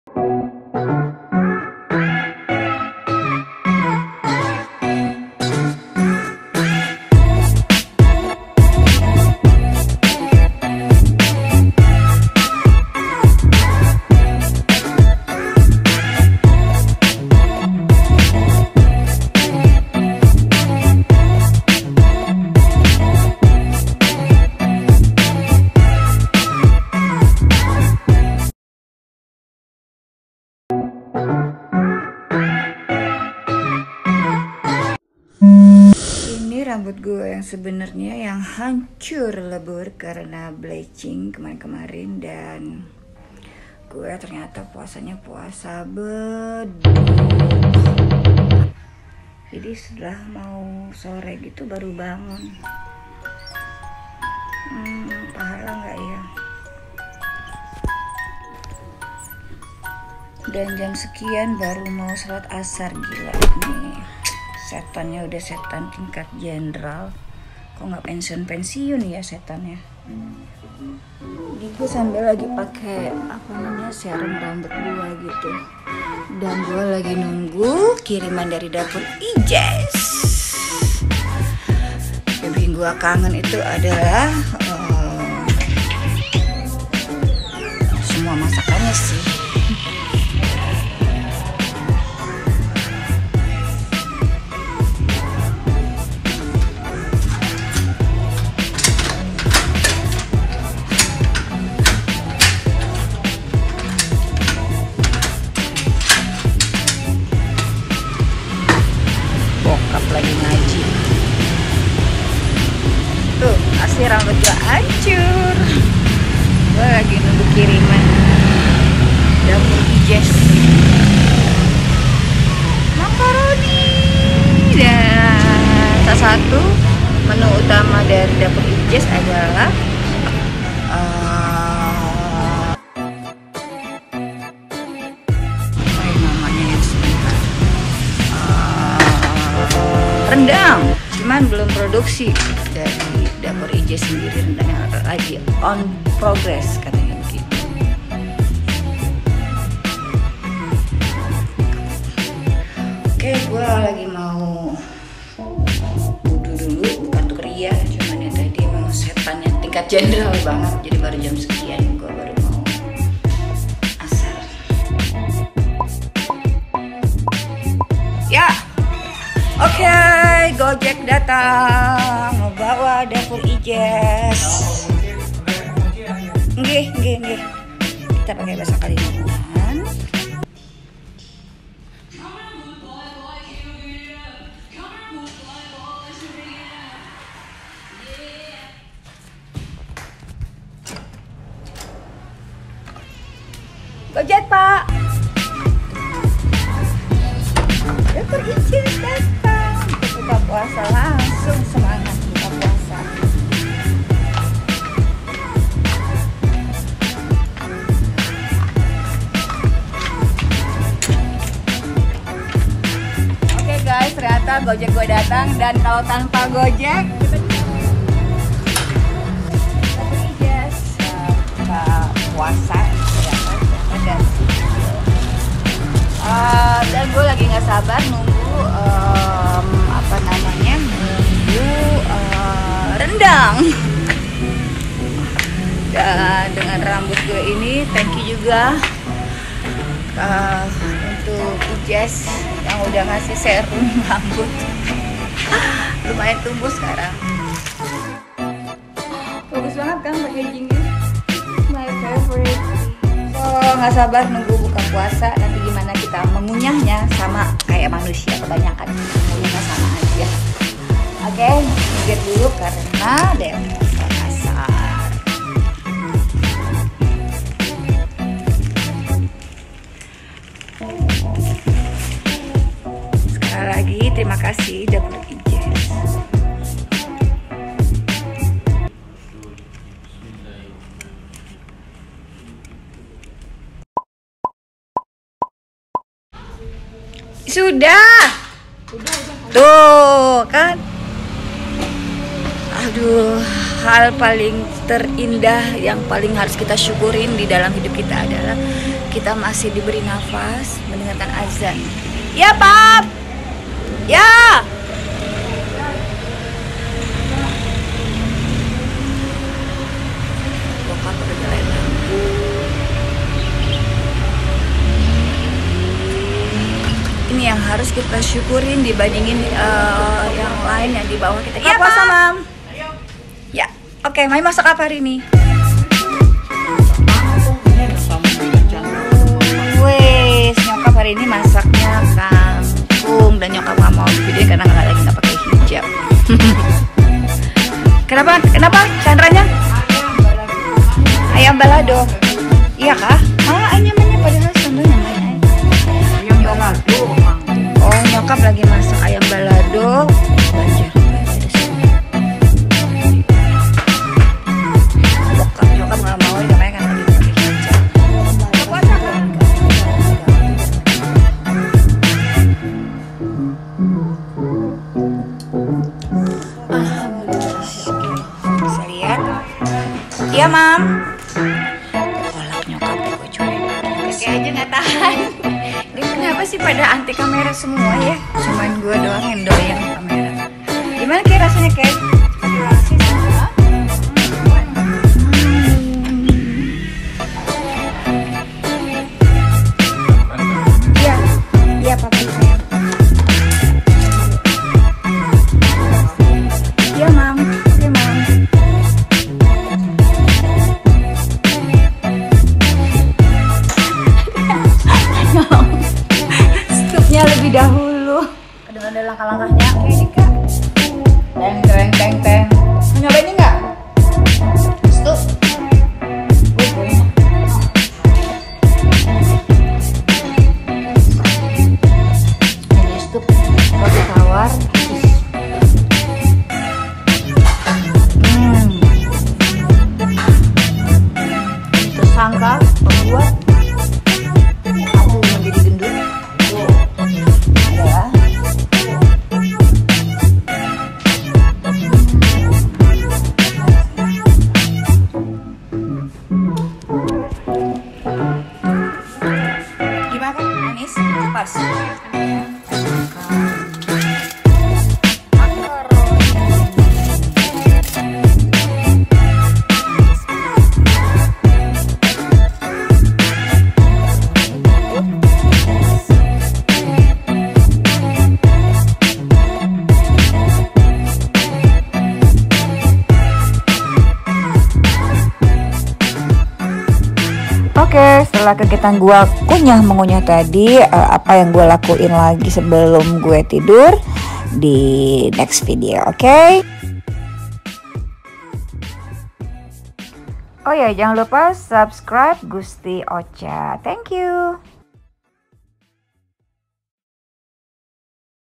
Daily routine during covid19 in Ramadhan
Daily routine during covid 19 in Ramadhan. This is  the real me without make up and my damaged hair... Bosan ga sih liat vlog yg menyajikan terlalu cantik ganteng paripurna? Kalau aku sih lagi suka ngeliat sesuatu  yg lebih real and le ih apa adanya... karena hidup dan manusia jauh dari kesempurnaan. Hanya Yg Di ATAS pemilik segala kesempurnaan.